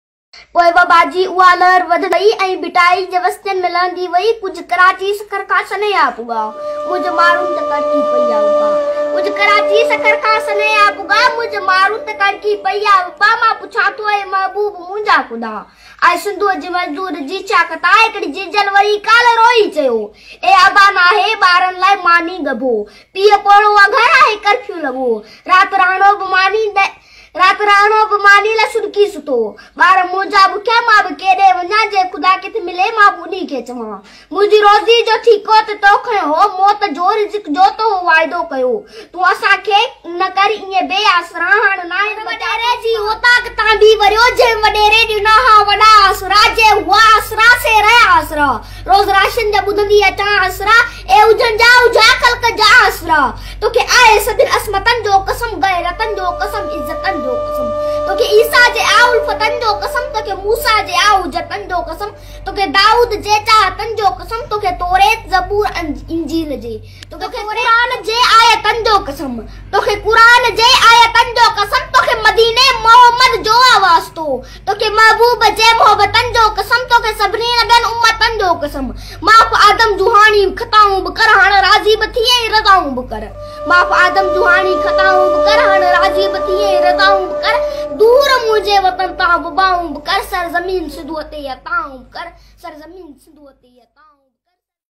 VOICE OF SINDH
Amzaing performance by a little girl of Sindh